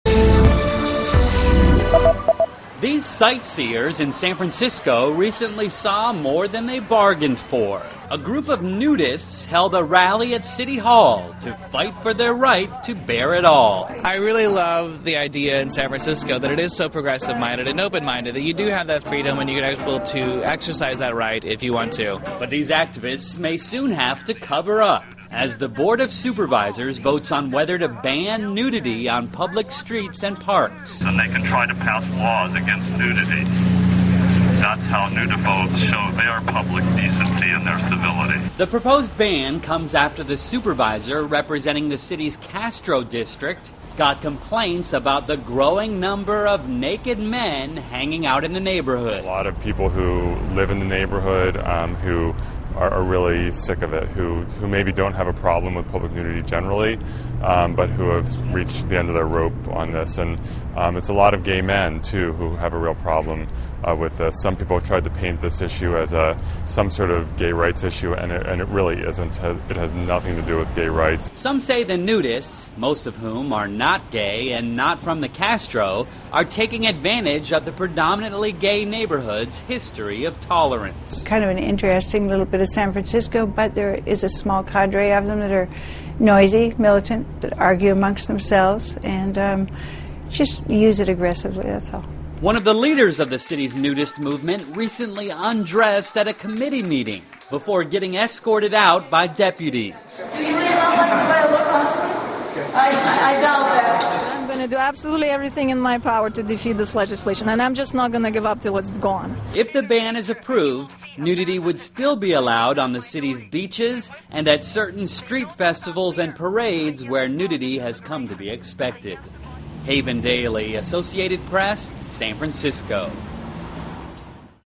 SF Supervisors Vote on Public Nudity Ban
The San Francisco Board of Supervisors votes Tuesday on whether to ban public nudity on streets, sidewalks and public parks. The proposal comes after complaints that nudity in the city's Castro District is getting out of control.  (Nov. 19)